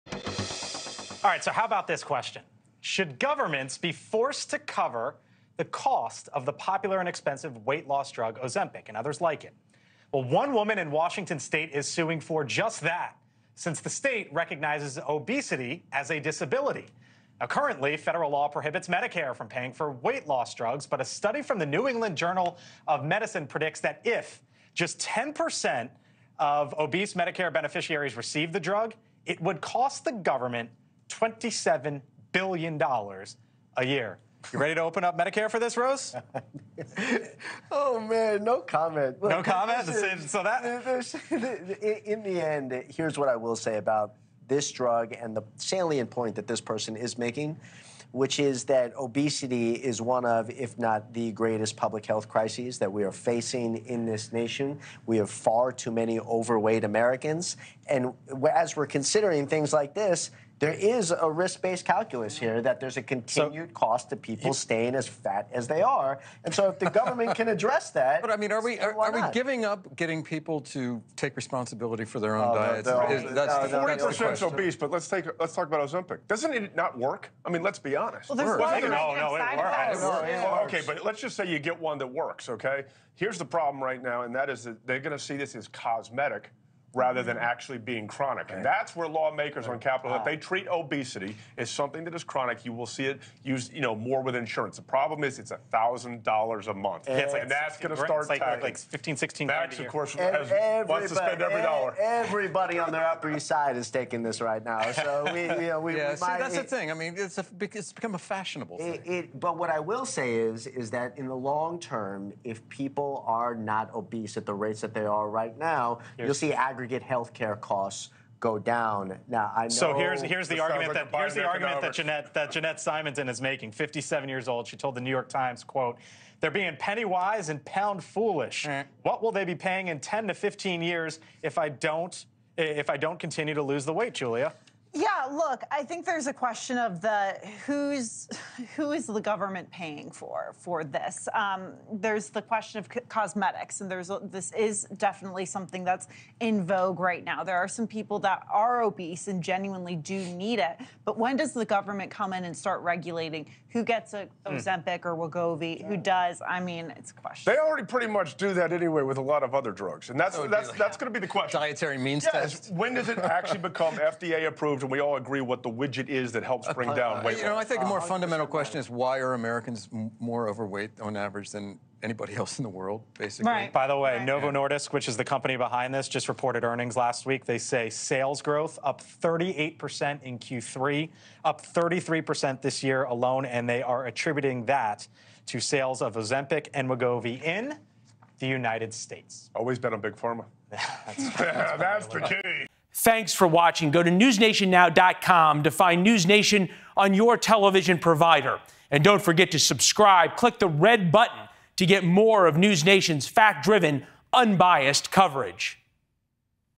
Should the government cover weight loss drugs like Ozempic? | The Hill
Should governments bear the cost of the popular and expensive weight loss drug Ozempic? One woman in Washington State is pursuing legal action to make it happen, citing the state's recognition of obesity as a disability.

Currently, federal law prohibits Medicare from covering weight loss medications, but a study published in the New England journal of Medicine predicts that if 10% of obese Medicare beneficiaries were to access the drug, it could cost the government $27 billion a year. "The Hill's" panel discusses the possibility of such a legislation.